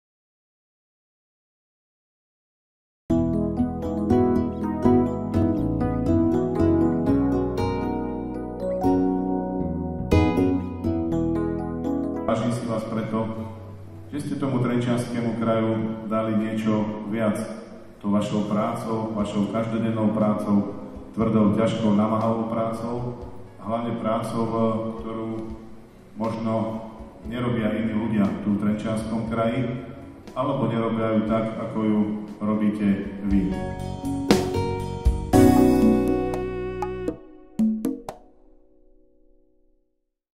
Slánostné odovzdávanie verejných uznaní TSK
Najvyššie ocenenie kraja v podobe Ceny TSK a Ceny predsedu TSK si odnieslo celkom 6 laureátov. V reprezentatívnych priestoroch Kasárne Trenčianskeho hradu boli ocenení Stanislav Liška, Peter Múčka, Imrich Tatiersky, ZUŠ Imra Weinera Kráľa, Ivana Marková a Kongregácia školských sestier de Notre Dame.